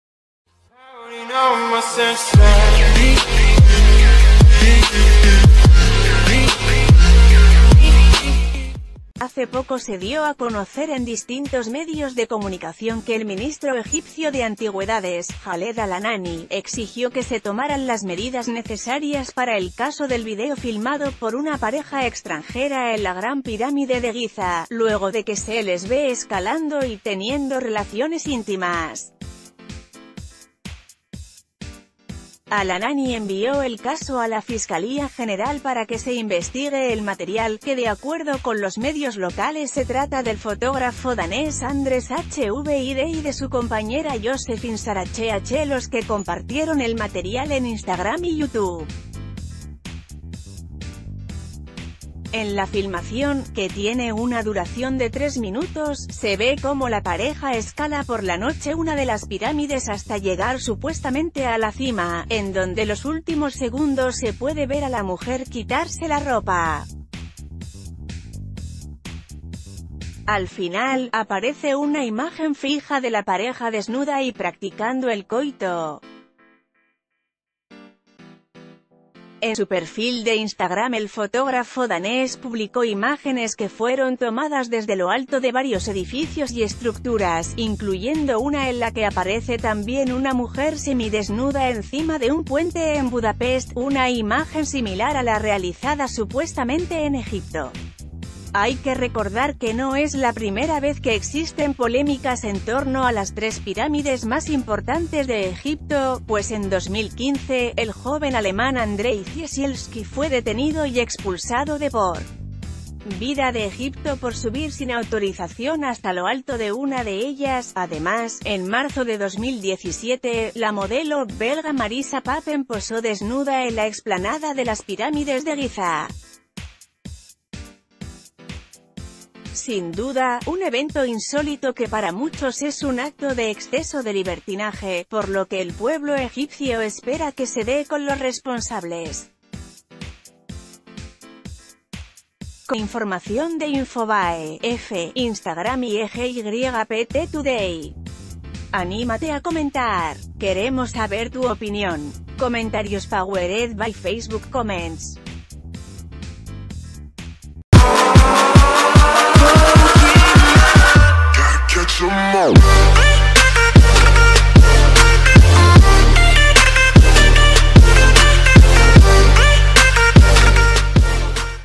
Escándalo: Captan a pareja teniendo intimidad sobre una pirámide en Egipto (FOTO)
Hace poco se dio a conocer en distintos medios de comunicación que el ministro egipcio de Antigüedades, Jaled al Anani, exigió que se tomaran las medidas necesarias para el caso del video filmado por una pareja extranjera en la Gran Pirámide de Guiza, luego de que se les ve escalando y teniendo rela...